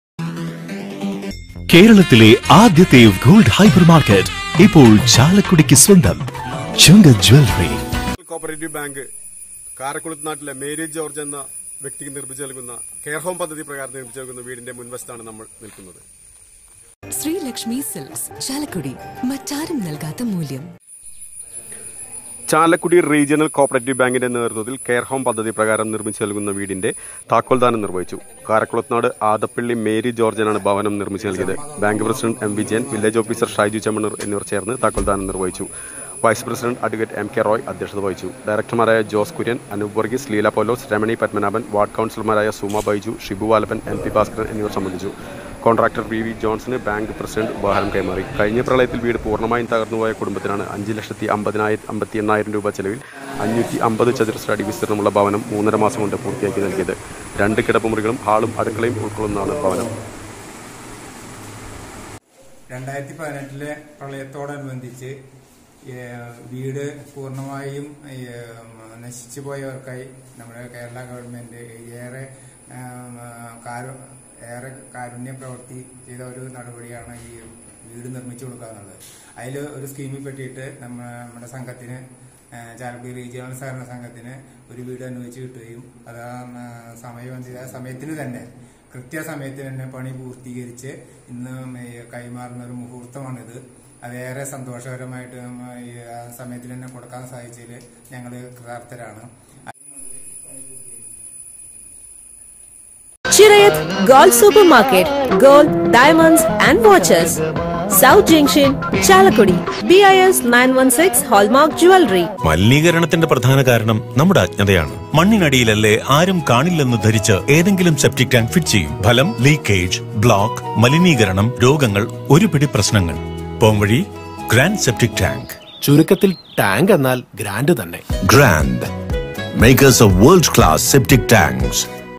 കെയർ ഹോം.താക്കോൽദാനം നടത്തി.
ചാലക്കുടി റീജിയണൽ കോപ്പറേറ്റീവ് ബാങ്ക് കേയർ ഹോം പദ്ധതി പ്രകാരം നിർമ്മിച്ച് നൽകിയ ഭവനത്തിന്റെ താക്കോൽദാനം നടത്തി.